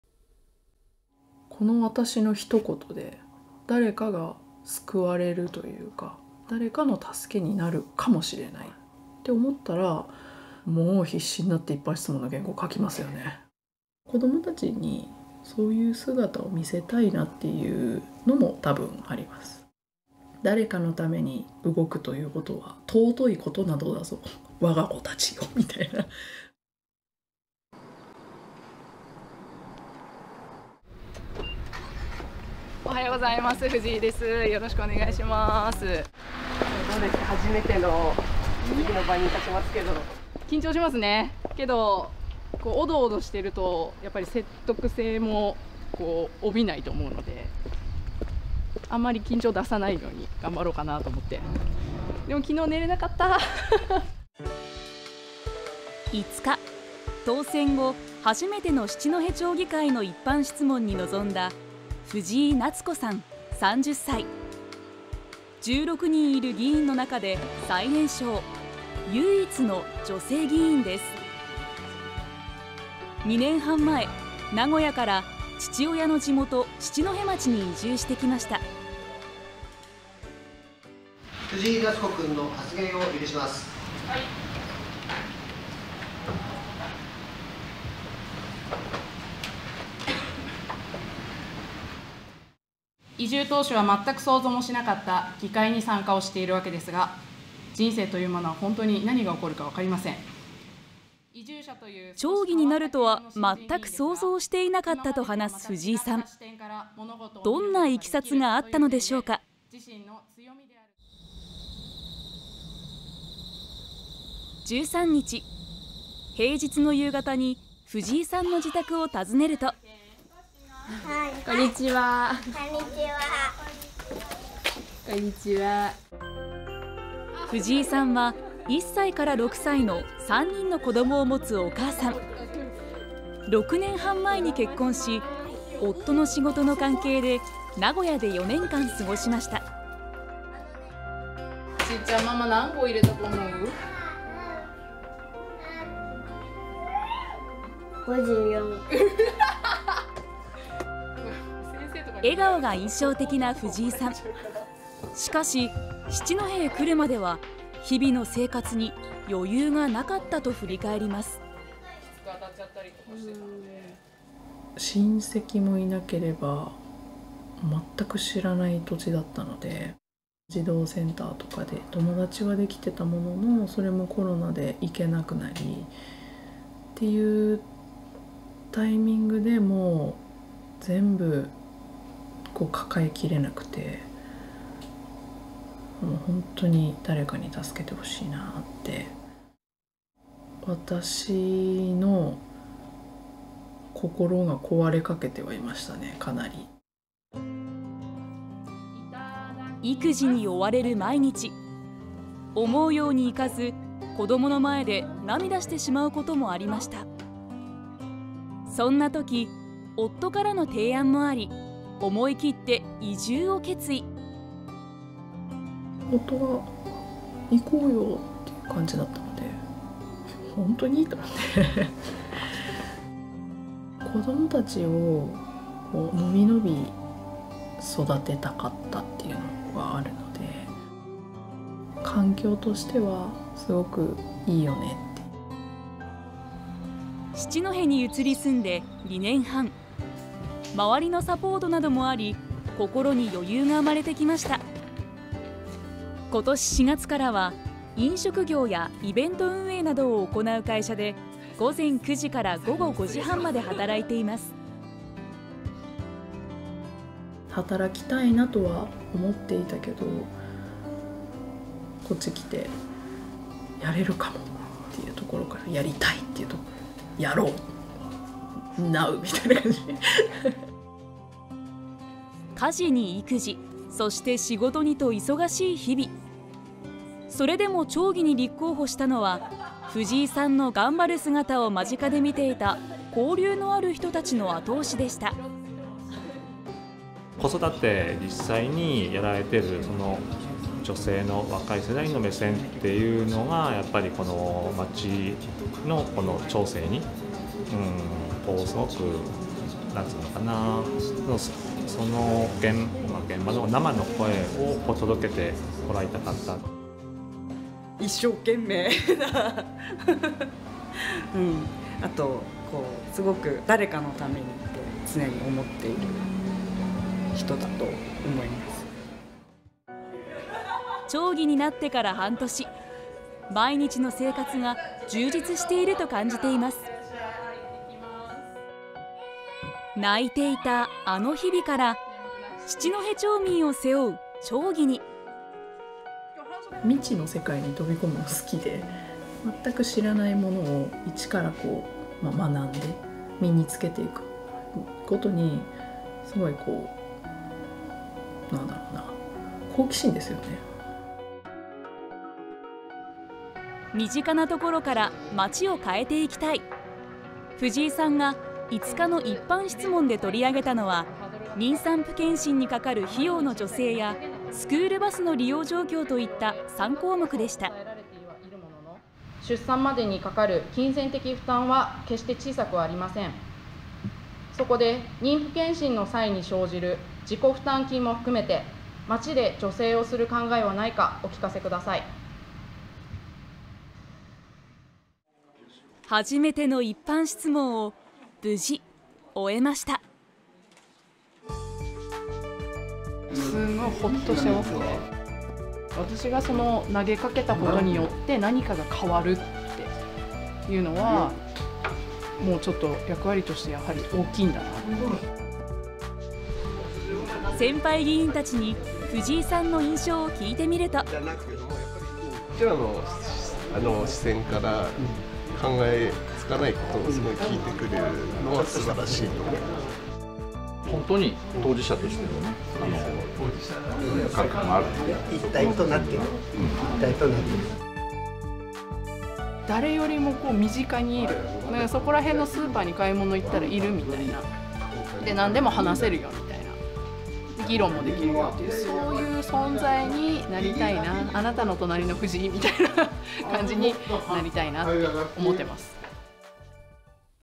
育児に仕事に奮闘 移住した町で町議になった30歳女性 初めての一般質問に密着「身近なところから変えていきたい」
４月に行われた青森県七戸町議会議員選挙で初挑戦ながらトップ当選を果たした藤井夏子さん。密着すると、昔の自分からの脱却と日々奮闘する姿がありました。

【藤井夏子さん】
「この私の一言で誰かが救われるというか、誰かの助けになるかもしれないって思ったら、もう必死になって一般質問の原稿書きますよね。子どもたちにそういう姿を見せたいなっていうのも多分あります」
「誰かのために動くことは尊いことなのだぞ我が子たちよ、みたいな」

【議場に到着した藤井夏子さん】
「おはようございます。藤井です。宜しくお願いします」

（Ｑ．初めて一般質問に立ちますが…）「緊張しますね。けど、おどおどしていると積極性も帯びないと思うので、あまり緊張を出さないように頑張りたいと思います。でもきのう寝られなかった～」

９月５日、当選後、初めての七戸町議会の一般質問に臨んだ藤井夏子さん、３０歳。１６人いる議員の中で最年少、唯一の女性議員です。２年半前、名古屋から父親の地元・七戸町に移住してきました。

【藤井夏子さん】
「移住当初は全く想像もしなかった議会に参加しているわけですが、人生というものは本当に何が起こるかわかりません」

町議になるとは、全く想像しなかったと話す藤井さん。どんないきさつがあったのでしょうか。

１３日、平日の夕方に藤井さんの自宅を訪ねると・・・

「こんにちは～」

藤井さんは１歳から６歳の３人の子どもを持つお母さん。７年前に結婚し、夫の仕事の関係で名古屋で４年間過ごしました。

「ちいちゃん、ママ何合入れたと思う？」

「５４！」

「ワハハハハハ」

笑顔が印象的な藤井さん。しかし、七戸へ来るまでは日々の生活に余裕がなかったと振り返ります。

【藤井夏子さん】
「親戚もいなければ全く知らない土地だったので」
「児童センターとかで友達は出来ていたものの、コロナで行けなくなりっていうタイミングで、全部抱えきれなくて。本当に誰かに助けてほしいなって」
「私の心が壊れかけてはいましたね。かなり」

育児に追われる毎日。思うようにいかず、子どもの前で涙してしまうこともありました。そんな時、夫からの提案もあり思い切って移住を決意。

【藤井夏子さん】
「夫が行こうよって感じだったので、本当に、って思って」
「子どもたちを伸び伸び育てたかったっていうのはあるので、環境としては良いよねって」

七戸に移り住んで２年半、周りのサポートなどもあり、心に余裕が生まれてきました。２０２３年４月からは飲食業やイベント運営などを行う会社で、午前９時から午後５時半まで働いています。

【藤井夏子さん】
「働きたいなとは思っていたけど、こっち来てやれるかも、やりたい、やろう、ＮＯＷみたいな感じ」

家事に育児、そして仕事にと忙しい日々。それでも町議に立候補したのは、藤井さんの頑張る姿を間近で見ていた交流のある人たちの後押しでした。

【田中清一さん】
「子育てを実際にやられている女性の若い目線っていうのが町の町政に、現場の生の声を届けてもらいたかった」

【田中千夏さん】
「一生懸命な。それで誰かのためにって常に思っている人だと思います」

町議になってから半年、毎日の生活が充実していると感じています。

泣いていたあの日々から、七戸町民を背負う町議に。

【藤井夏子さん】
「未知の世界に踏み込むのが好きで、全く知らないものを一から学んで身に着けていくことに、好奇心ですよね」

身近なところから町を変えていきたい。藤井さんが５日の一般質問で取り上げたのは、妊産婦検診にかかる費用の助成や、スクールバスの利用状況といった３項目でした。

「出産までにかかる金銭的負担は決して小さくありません。そこで妊婦検診の際に生じる自己負担金も含めて、町で助成をする考えはないかお聞かせください」

初めての一般質問を無事終えました。

【藤井夏子さん】
「すごいホッとしていますね」
「私が投げかけたことで、何かが変わるっていうのは、役割としては大きいんだなって」

先輩議員たちに藤井さんの印象を聞いてみると・・

【先輩町議】
「私たちの視点からは考えつかないことを聞いてくれるのは素晴らしいと思います」

「本当に当事者としての感覚があるので、良いと思います」

【藤井夏子さん】
「誰よりも身近にいる、そこら辺のスーパーに行ったらいるみたいな、何でも話せるよみたいな、議論も出来るよっていう。そういう存在になりたいな。あなたの隣の藤井みたいな。そういう存在になりたいなって思っています」